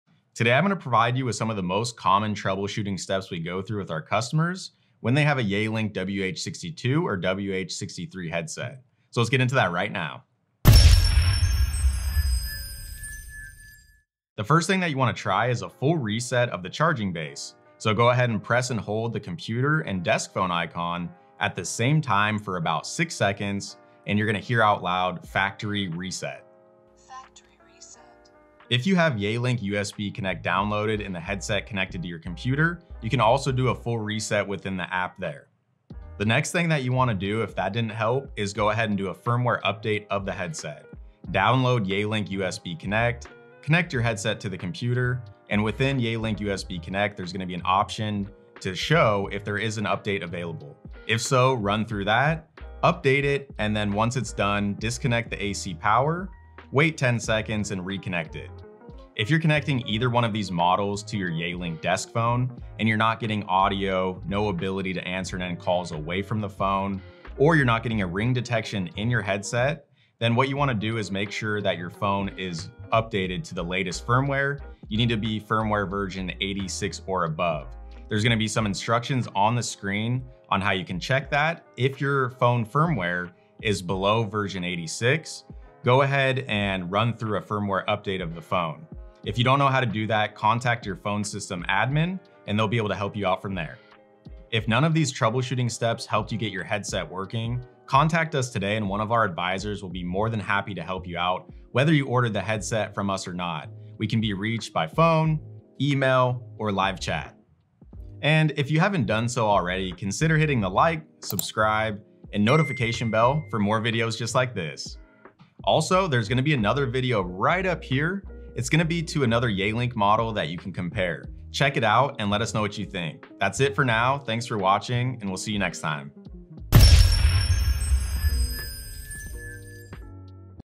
Yealink WH62/WH63 Troubleshooting Guide
Want MORE noise cancellation? Try neep noise cancelling software free

Join Drew as he goes over some troubleshooting tips for your Yealink WH62 and WH63 wireless headsets.

🟣Featured Product🟣

JH 4hrs